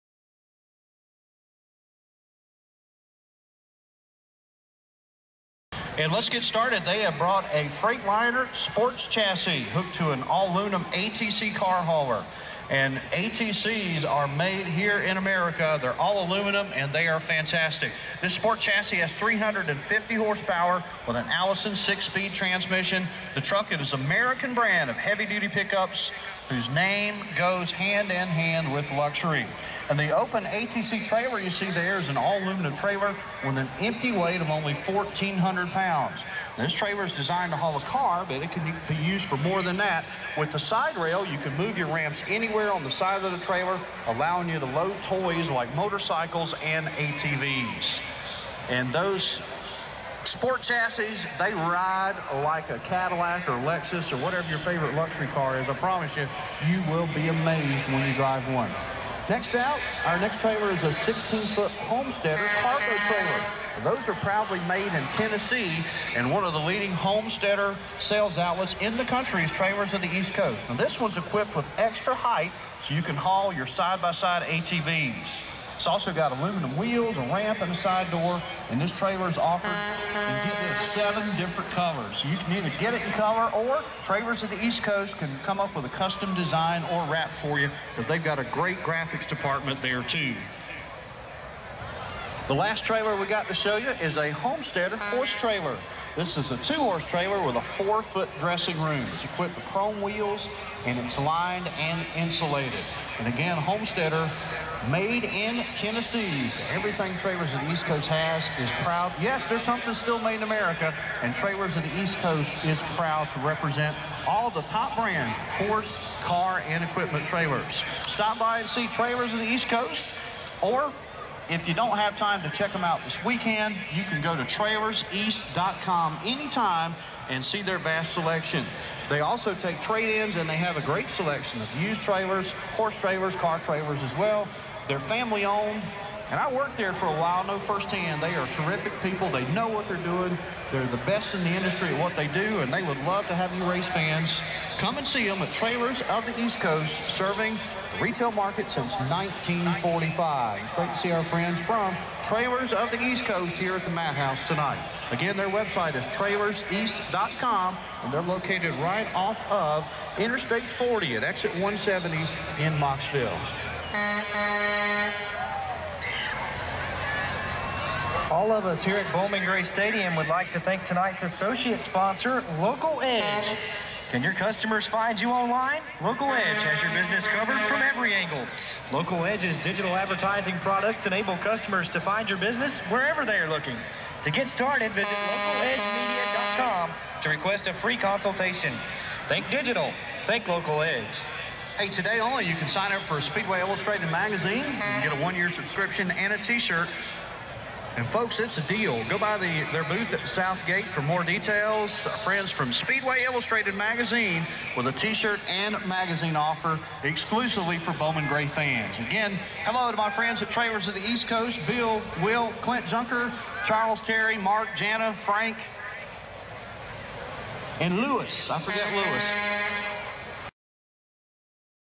Historic Bowman Gray Stadium Hosts Parade of Trailers
Trailers of the East Coast had the privilege of running a "Parade of Trailers" at historic Bowman Gray Stadium in Winston Salem, NC on race night, Saturday, June 25, 2016.

Featured on the show “Madhouse” on the History Channel, Bowman Gray is NASCAR’s oldest track, a ¼ mile oval hosting weekly stock, modified & sportsman car races since 1949.

A big shout out to Bowman Gray Stadium and thank you from Trailers of the East Coast.  It was a blast taking to the track of a real NASCAR legend, the track at Bowman Gray Stadium!